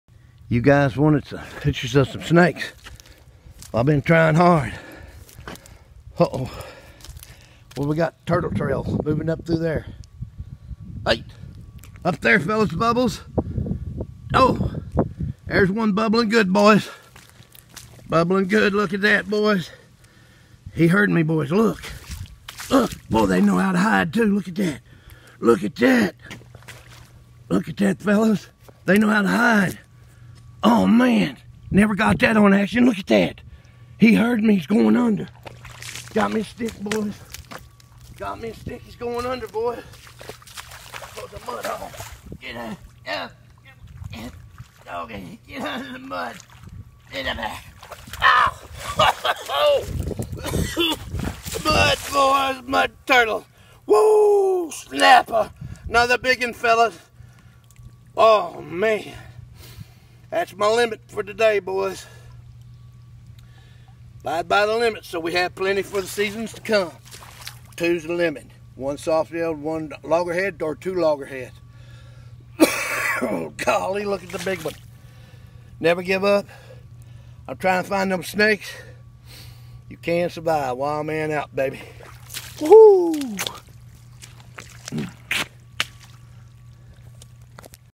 Looking for snakes
Why looking for snakes I found food baby never give up you can survive wildman out